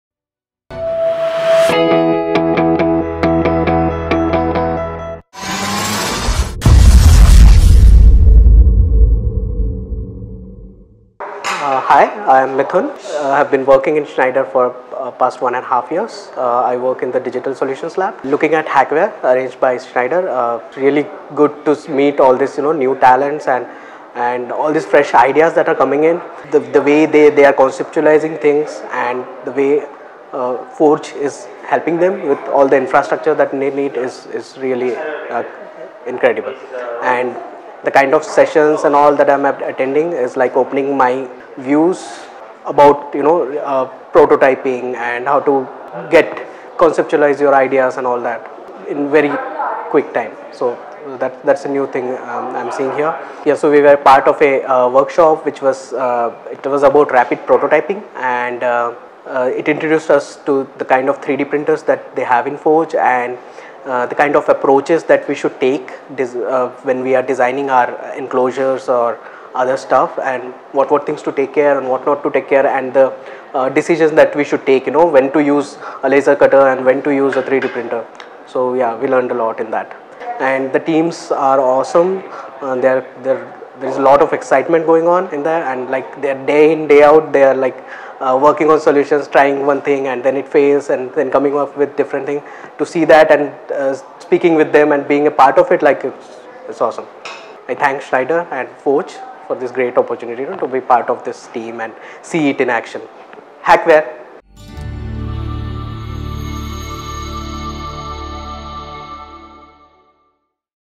Hackware Testimonial - Mithun, Schneider Electric.
"It's good to meet the new talents and all the fresh new ideas that are coming in. The way the participants are perceiving ideas and the way Forge is helping them with the infrastructure they need is incredible. Hackshops broadened my knowledge of prototyping and conceptualising ideas into a product in a short time. The rapid prototyping Hack shop introduced us to the kind of 3D printers at Forge and the rapid prototyping techniques. It also helped us understand when to use a 3D printer and when to use a laser cutter. "- Mithun, Schneider Electric.